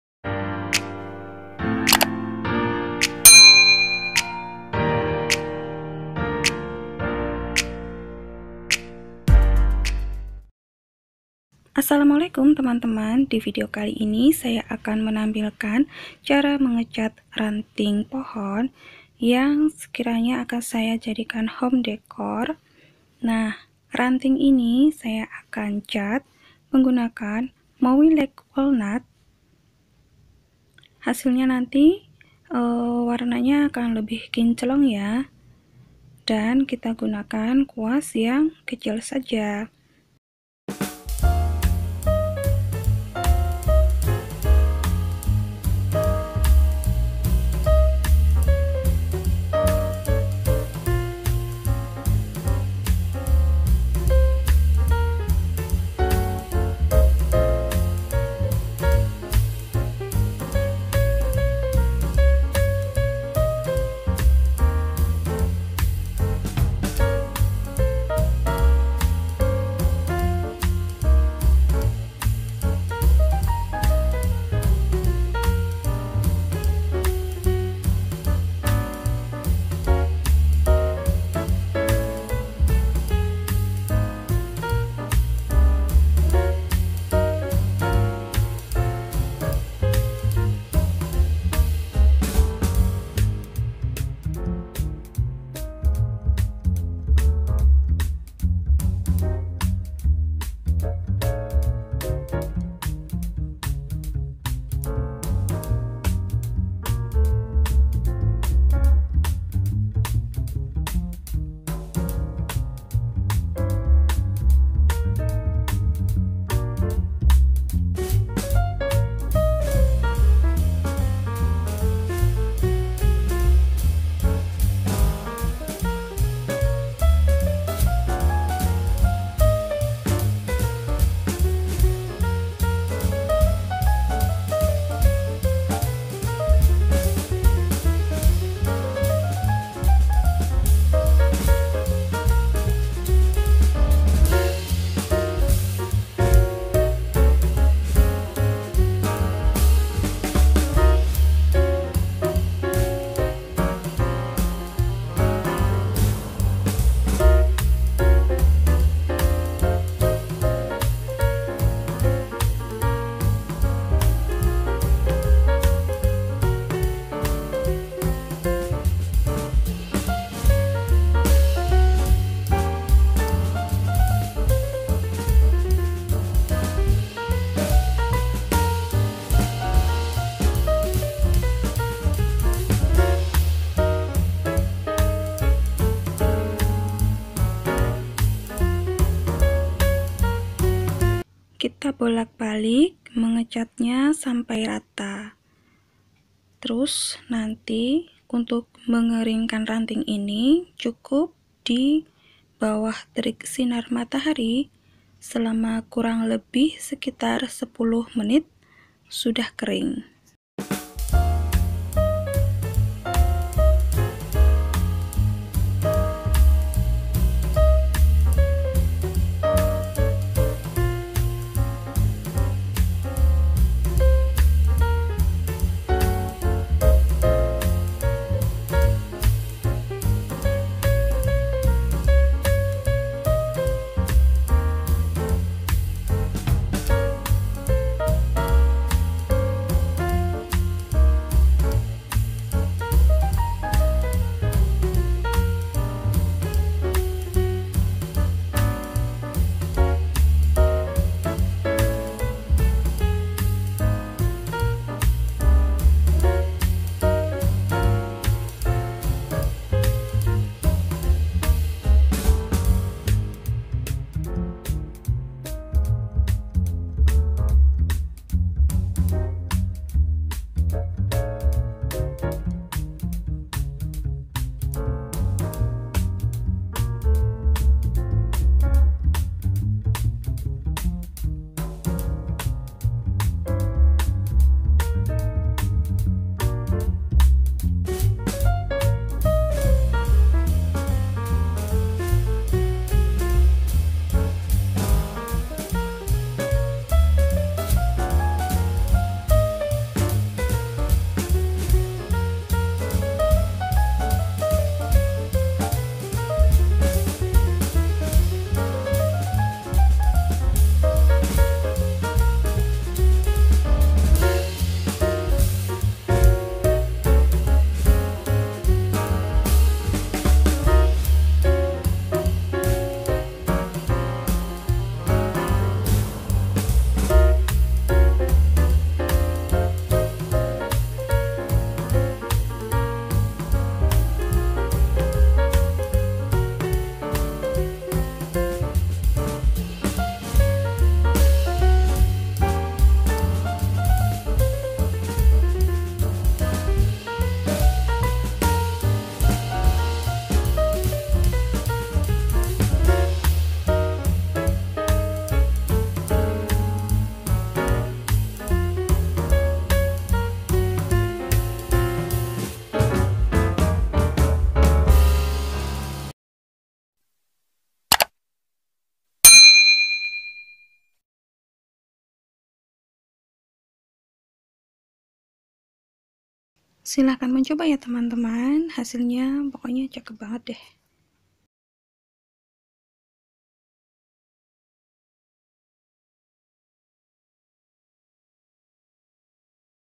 Cara Mengecat Ranting Kayu untuk Homedecor
Assalamualaikum teman-teman
Di video kali ini saya akan memperlihatkan bagaimana cara mengecat ranting kayu menggunakan cat Mowilex Woodstain Walnut 503.
Hasil akhirnya sangat bagus untuk mempertajam warna asli ranting kayu yang akan saya manfaatkan sebagai hiasan homedecor.
Jika ada pertanyaan silakan tulis di kolom komentar. Selamat mencoba di rumah. Semoga bermanfaat.